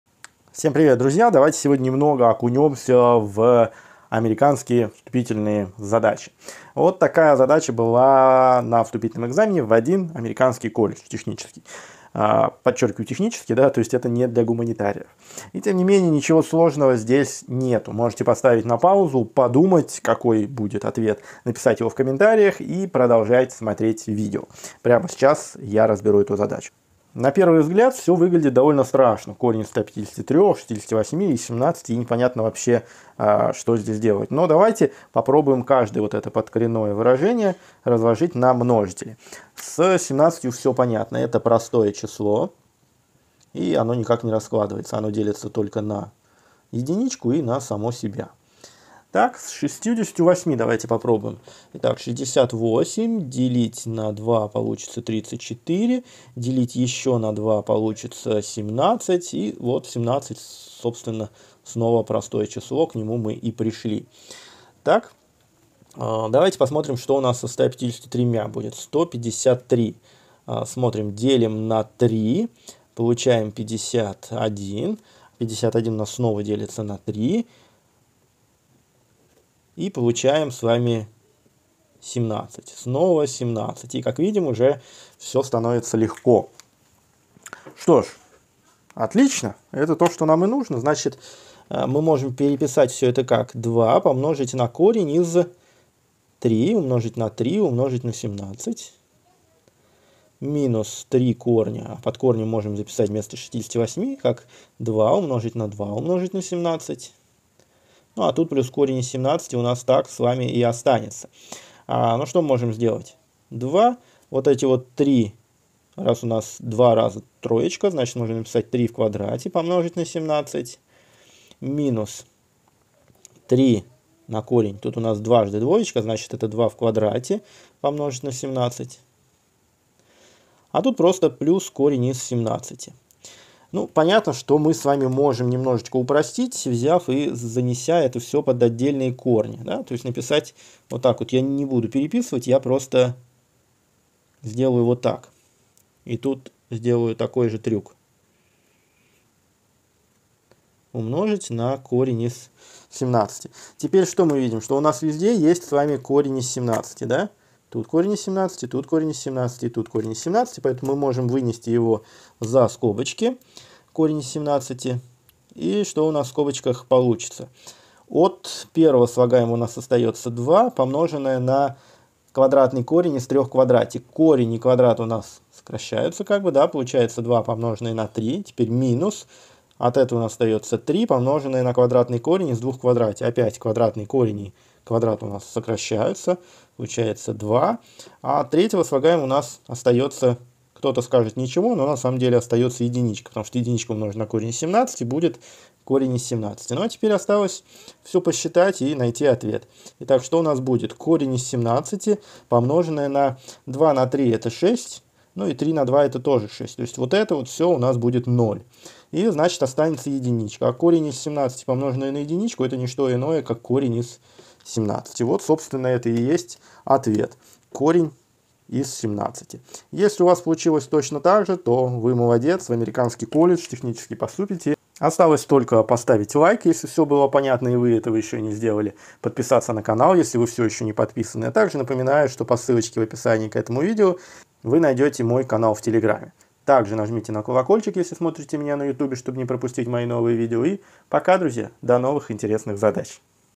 Задача из вступительного в американский колледж
2√153 - 3√68 + √17 = ?